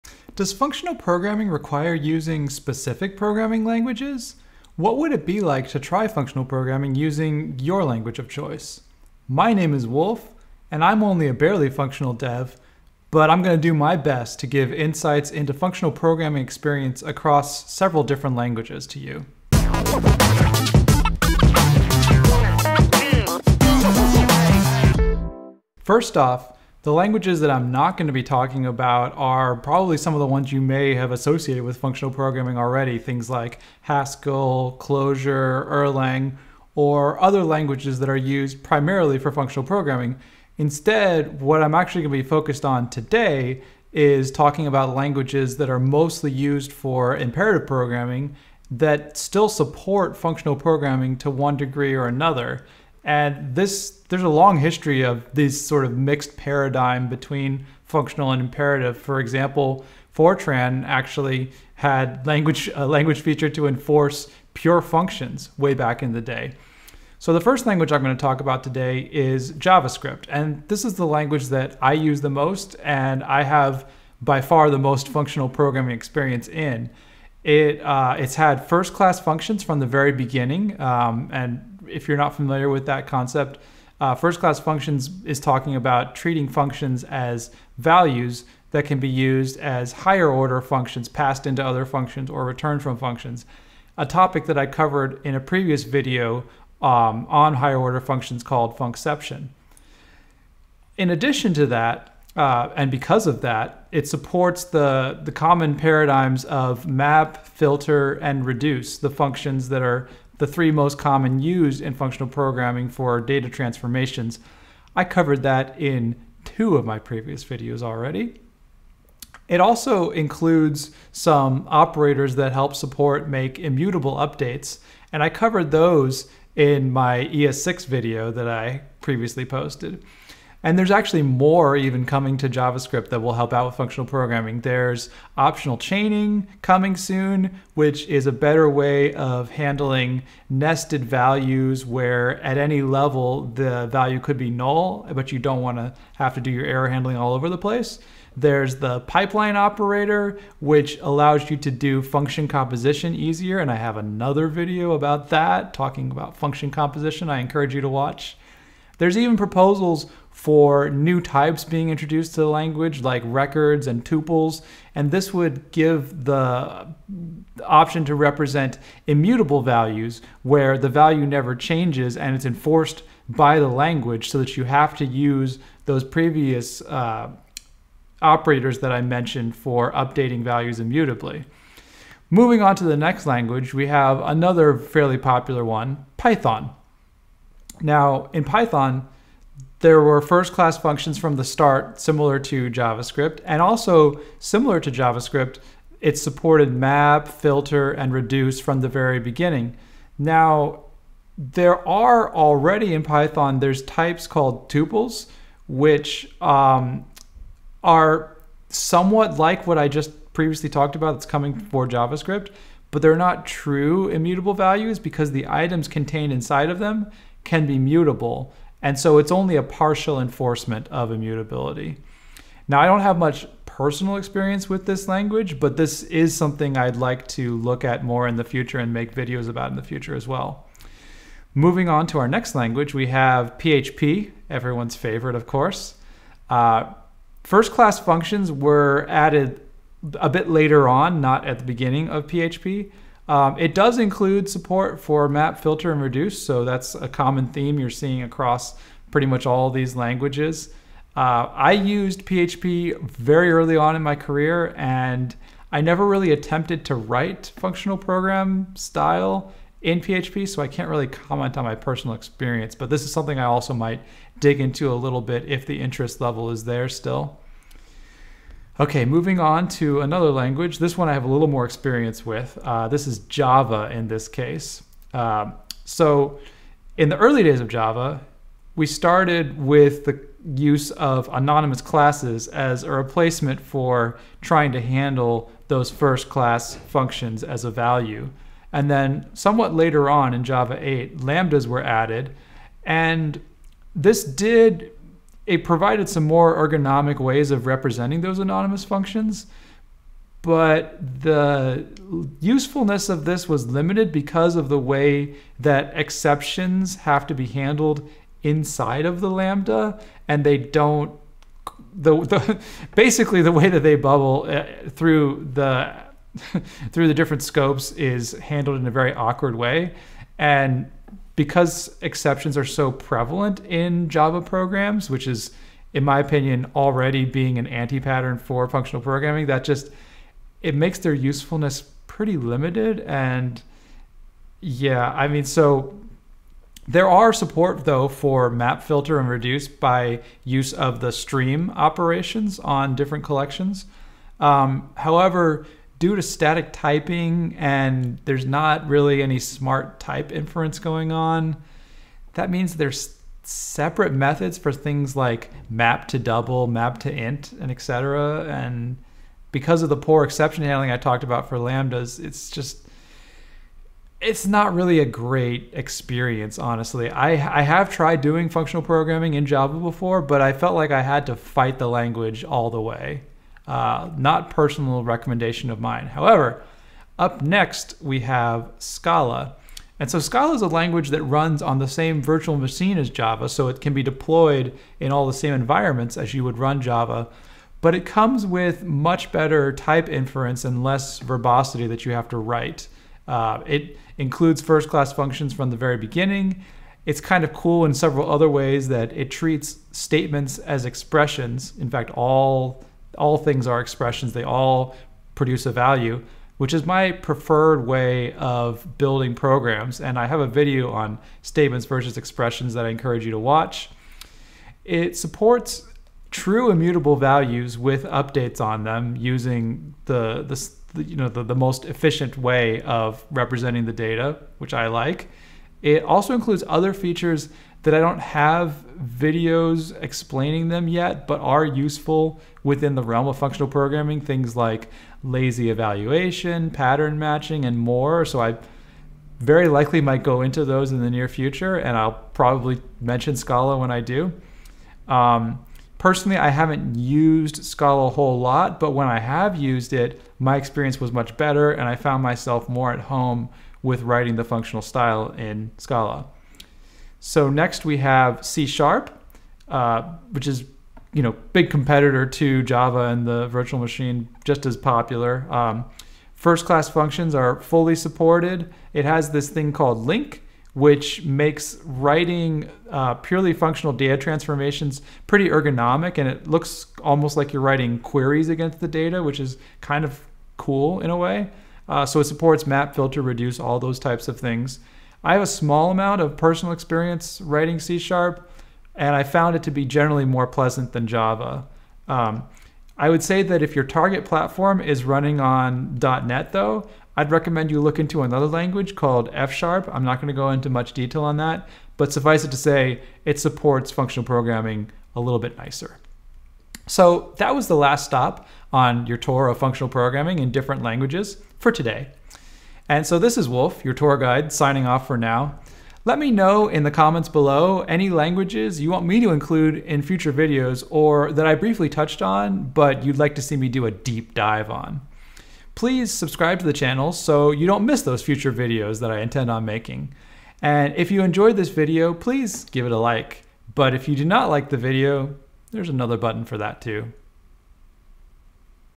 functional programming languages [episode 11]
my personal experiences and insights with functional programming in languages that support a hybrid of imperative and functional programming:
- JavaScript
- Python
- PHP
- Java
- Scala
- C#

0:00 intro
0:25 languages not included
1:07 javascript
3:02 python
3:51 php
4:29 java
6:17 scala
7:37 c sharp
8:26 ending